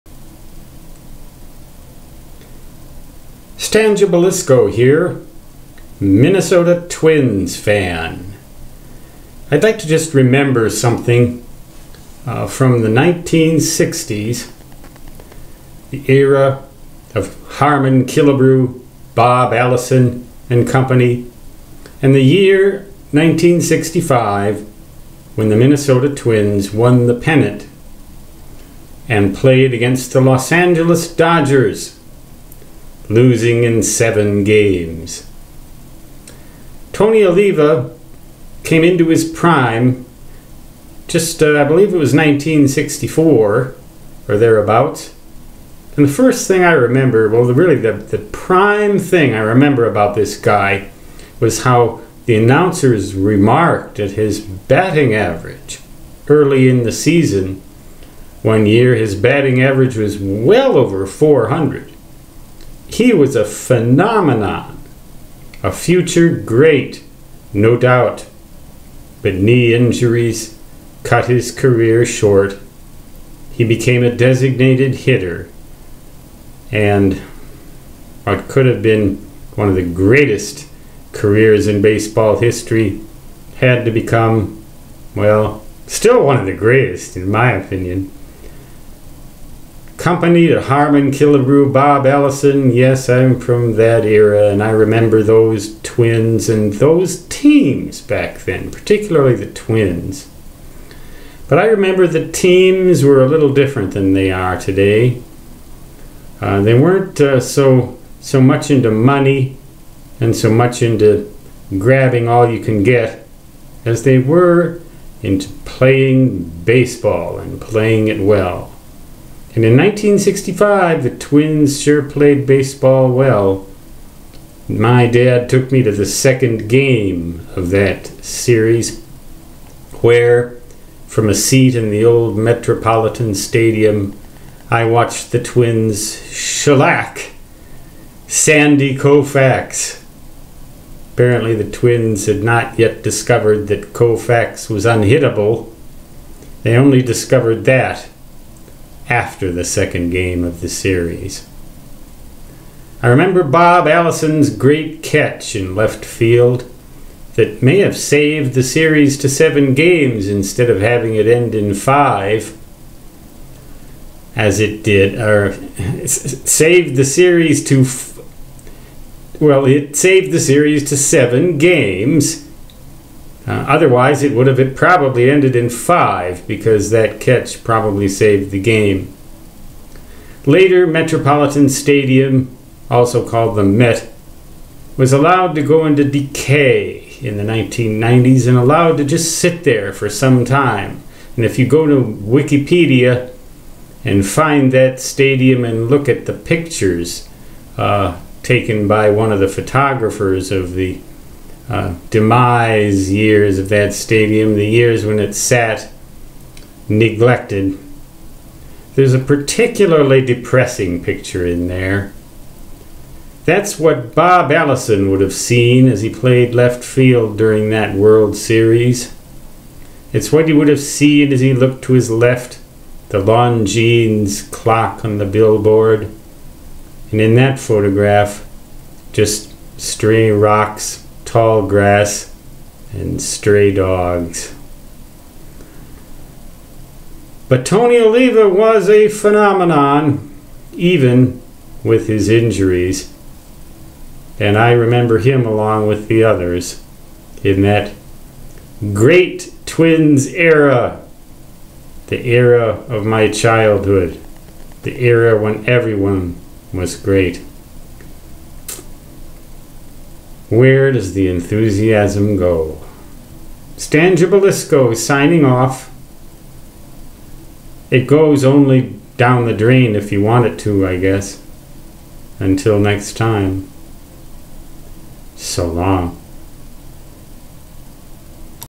Remembering Tony Oliva
"Tony O" was part of my childhood, the Twins of the Met.